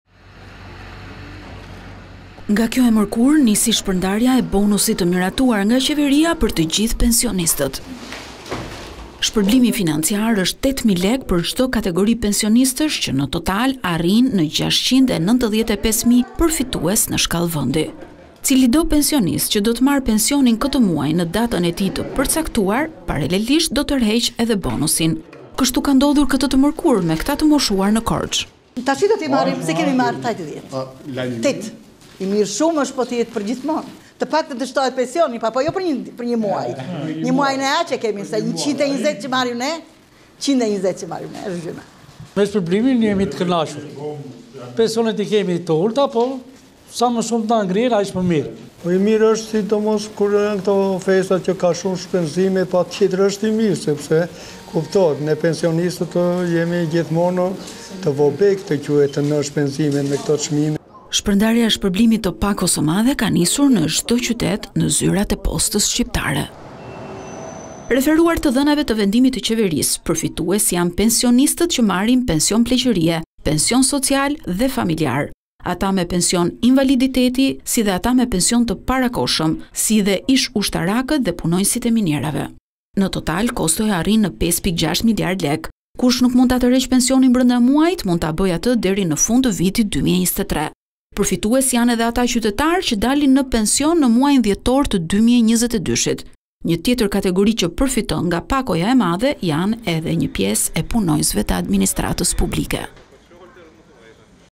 Nis shpërndarja e bonusit të qeverisë; 8 mijë lekë për çdo pensionist dhe invalidët - Vizion Plus
Nga kjo e mërkurë nisi shpërndarja e bonusit të miratuar nga qeveria për të gjithë pensionistët.Shpërblimi financiar është 8 mijë lekë për cdo kategori pensionistësh, që në total arrijnë në 695 mijë përfitues në shkallë vëndi.Cilido pensionist që do të marrë pensionin këtë muaj në datën e tij të përcaktuar, paralelisht do të tërheqë edhe bonusin.Kështu ka ndodhur me të moshuarit në Korcë dhe në Fier pensionistët kanë marrë shpërmblimn e pakos se madhe.Referuar të dhënave të vendimit të qeverisë përfitues janë pensionistët që marrin pension pleqërie, pension social dhe famijarë, ata me pension invaliditeti, ata me pension të parakohshëm si dhe ish ushtarakë dhe punonjësit e minierave.Në total kostoja arrin në 5.6 miliard lekë. Kush nuk mund ta tërheqë pensionin brenda muajit, mund ta bëjë deri në fund të vitit 2023. Përfitues janë edhe ata qytetarë që dalin në pension në muajin dhjetor 2022. Një tjetër kategori që përfiton nga pakoja e madhe janë edhe punonjësit e administratrës.